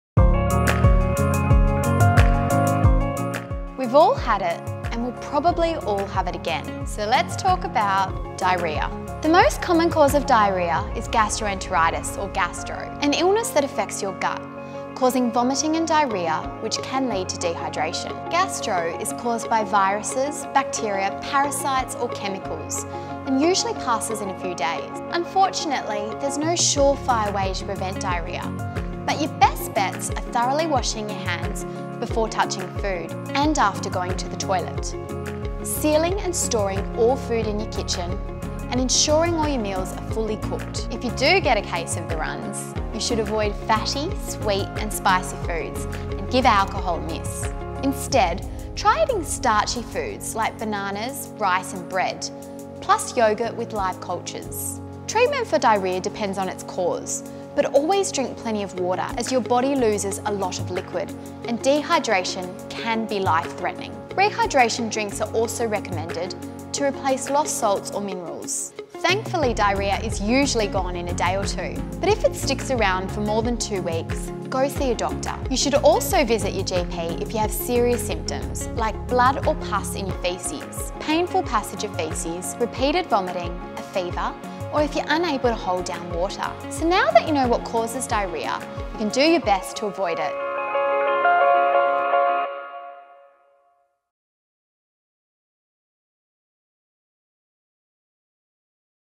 What causes diarrhoea? Experts explain
Why do we sometimes get diarrhoea? And what can make it stop? Medibank expert Dr Zoe Boyatzis explains what can cause diarrhoea, as well as food to eat (and avoid) when you’ve got “the runs”.